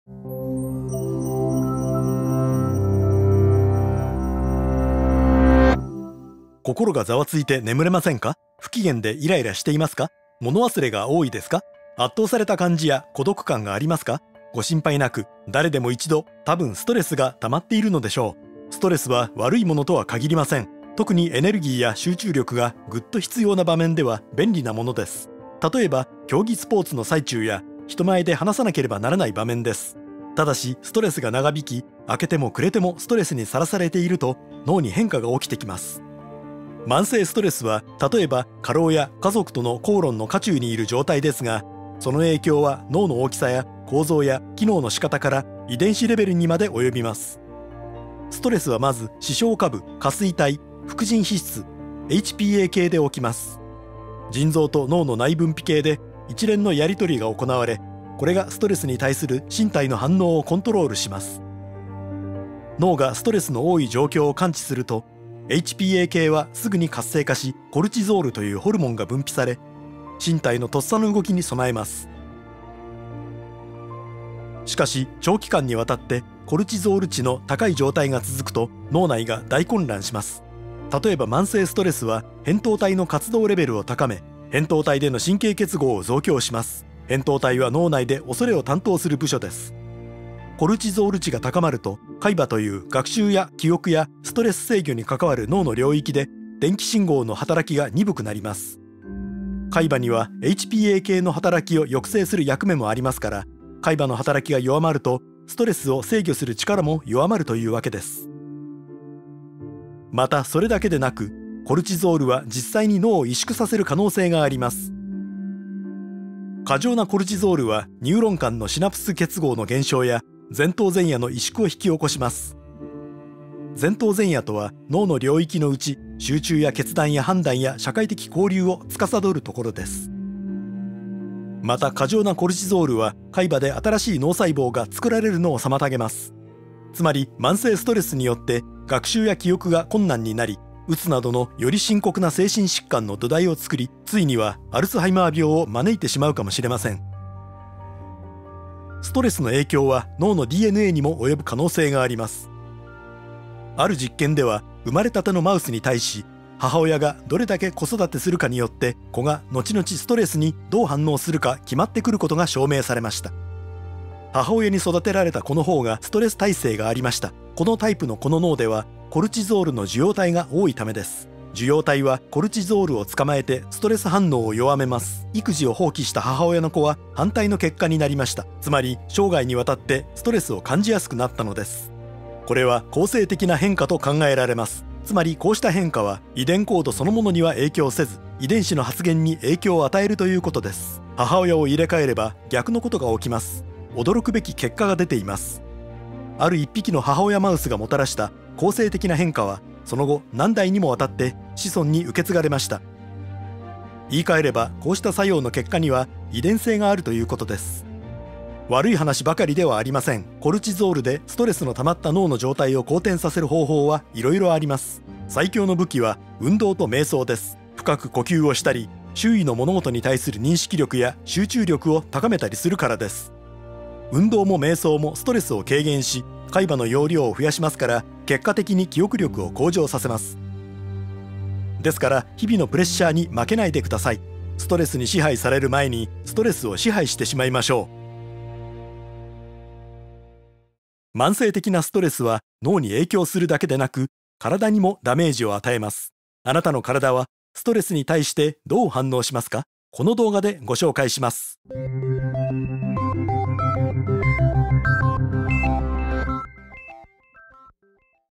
ストレスが身体に及ぼす影響 ― マデュミタ・ムルジア
ストレスは悪いこととは限りません。たとえば競技スポーツの最中や人前で話すというような、エネルギーや集中力を一気に必要とする場面で、ストレスは便利だったりします。
ただしストレスがいつまでも続くと、それによって脳が変わってきてしまいます。マデュミタ・ムルジアが、脳の大きさや構造、機能から遺伝子レベルにまで及ぶ慢性ストレスによる脳への影響を紹介します。

Madhumita Murgia によるレッスン、Andrew Zimbelman 監督。